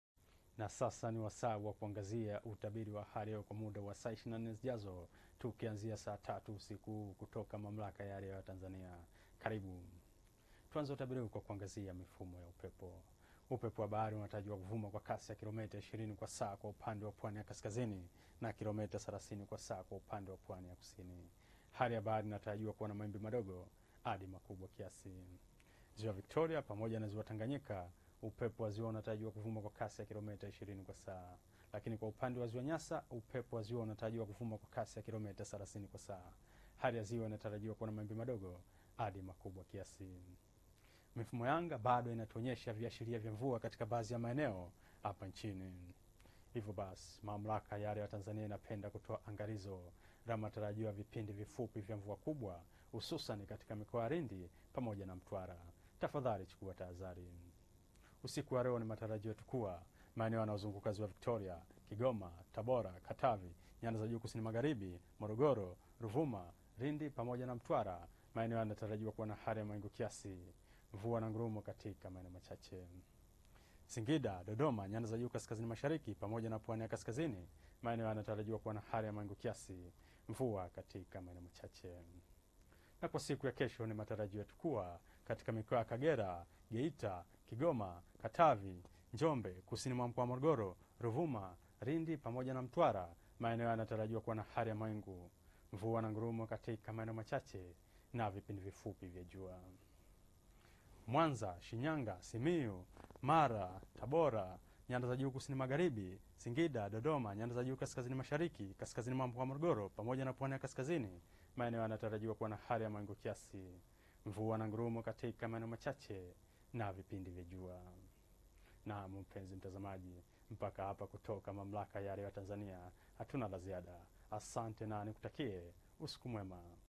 UTABIRI WA HALI YA HEWA LEO USIKU 26.03.2024
Utabiri wa Hali ya Hewa wa saa 24 zijazo kuanzia saa 3 usiku wa leo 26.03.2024 unaletwa kwenu na Mchambuzi wa Hali ya Hewa; Mohamed Hamisi.